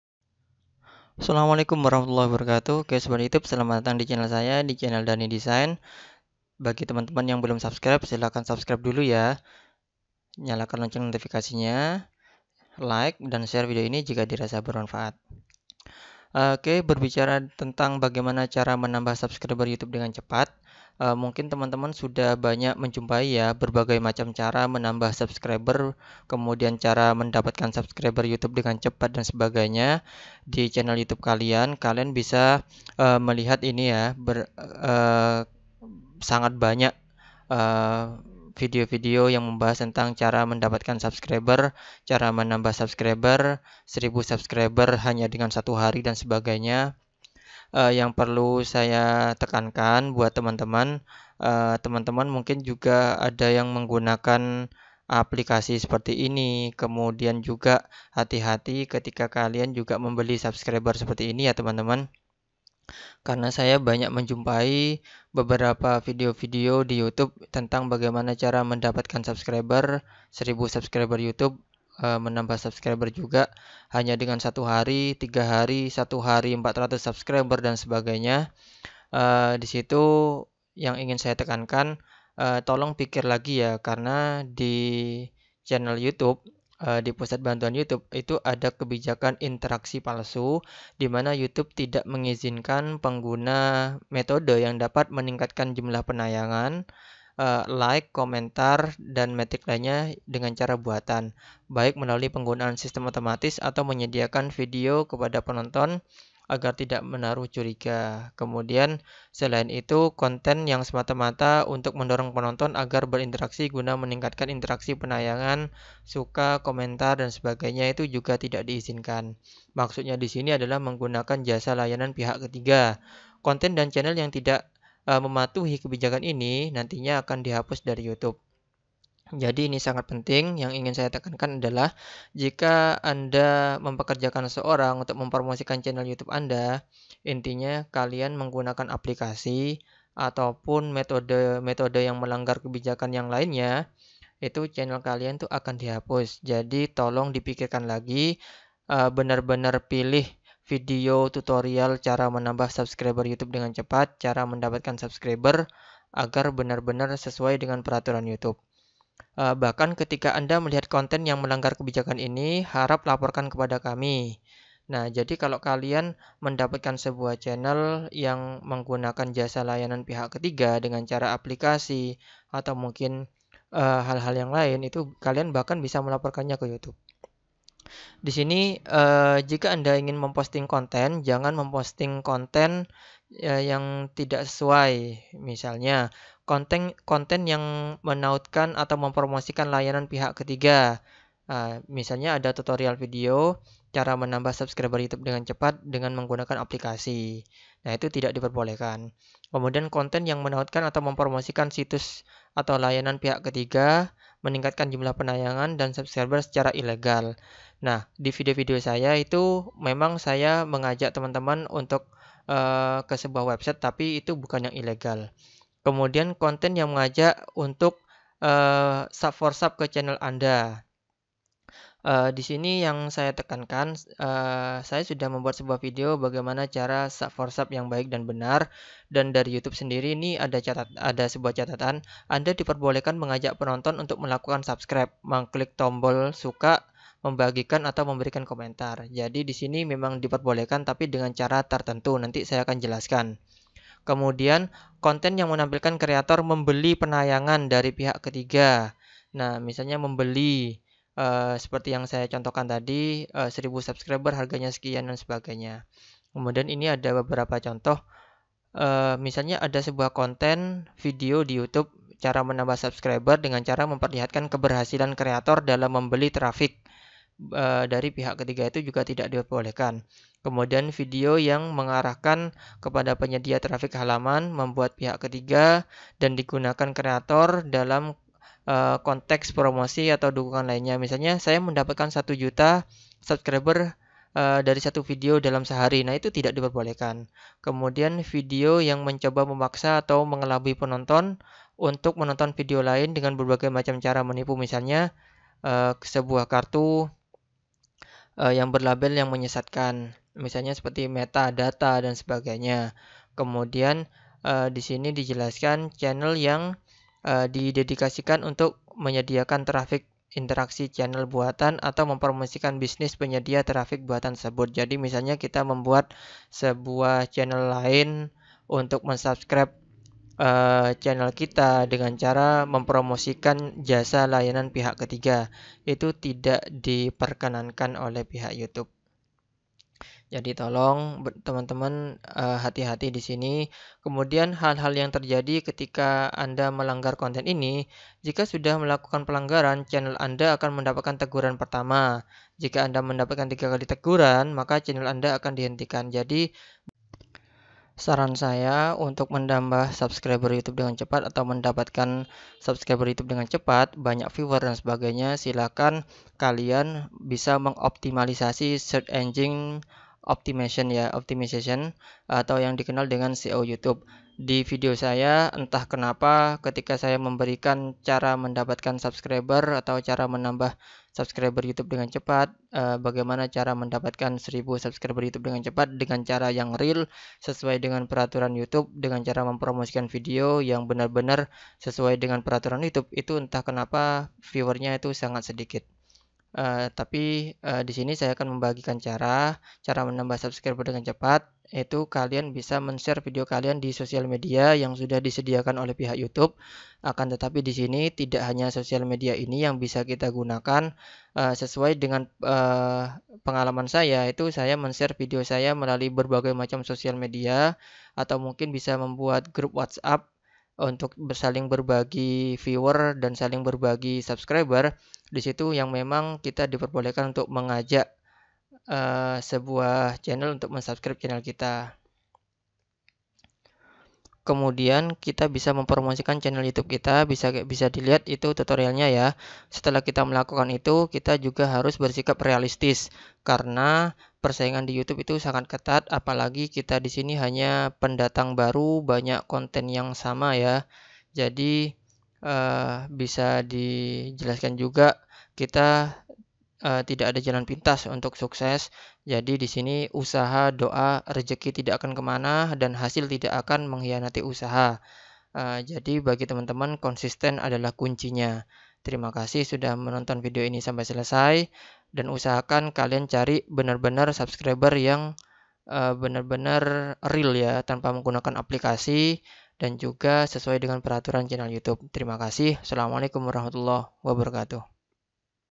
Cara mendapatkan 1000 Subscriber Youtube Tanpa Aplikasi
cara menambah subscriber youtube dengan cepat tanpa aplikasi dan sesuai peraturan youtube.